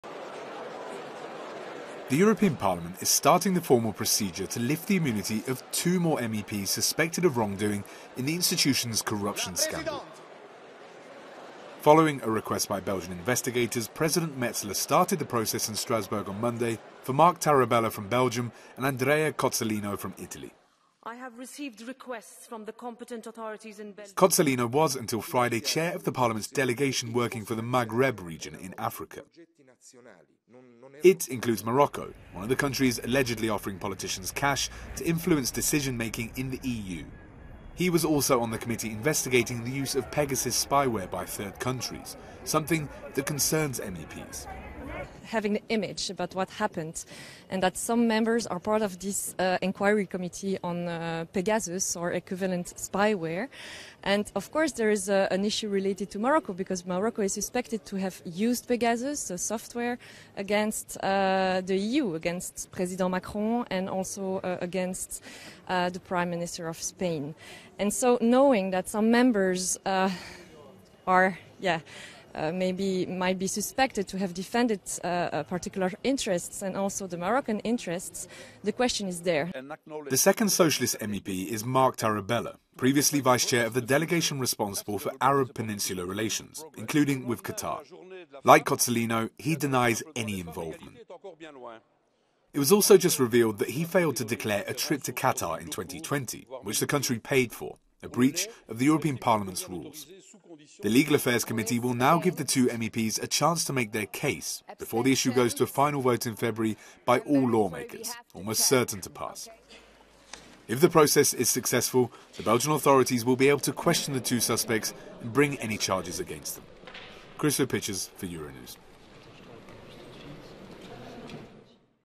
EU corruption scandal: European Parliament starts process to waive immunity of two more MEPs
Marc Tarabella and Andrea Cozzolino are both members of the Socialists & Democrats Group.